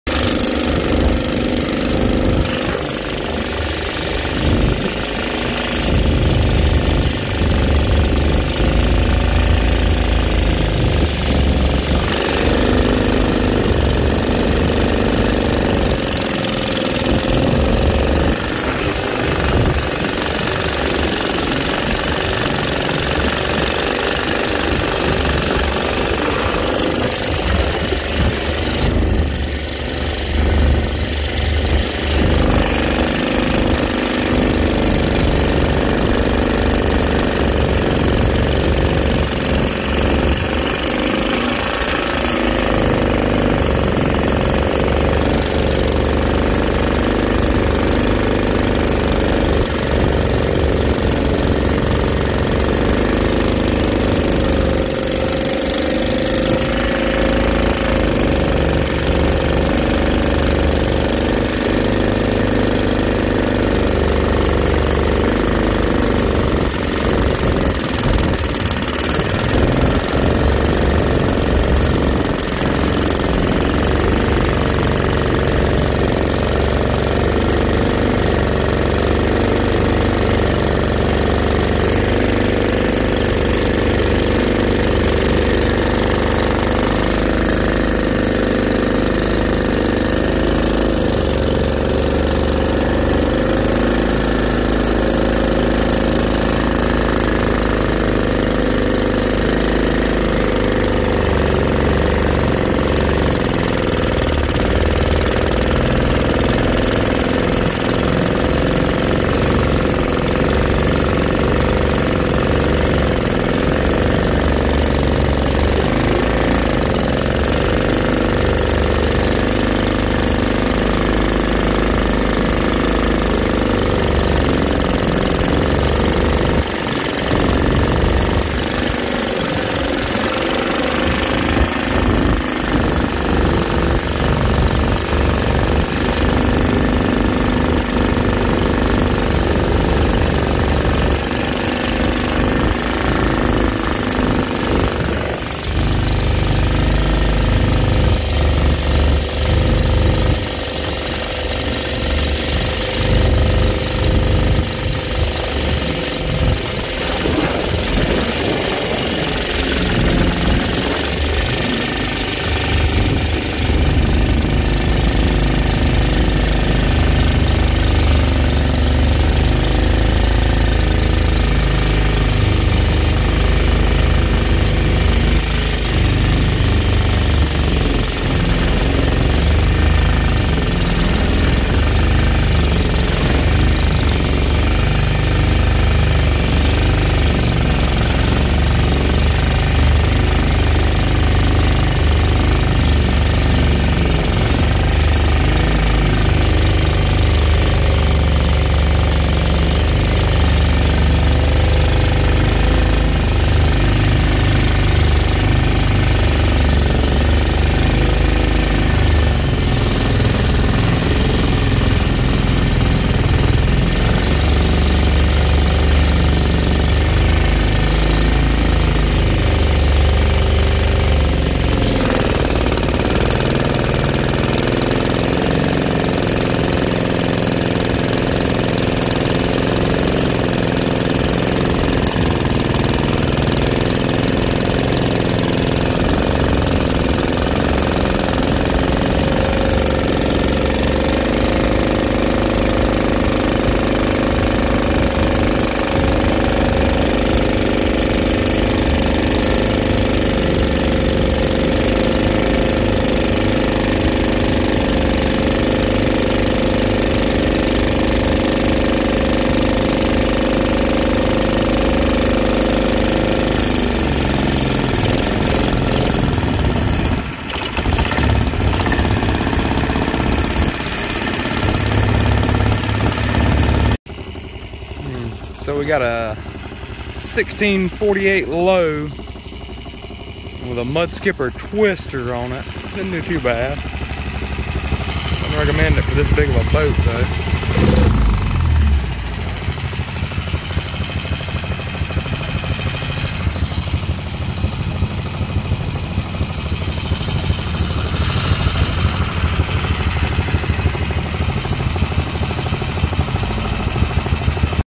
Mud Skipper Twister on a 1648 lowe
mud Skipper Twister 5 hp worn out gx160 pushing my flat bottom lowe 1648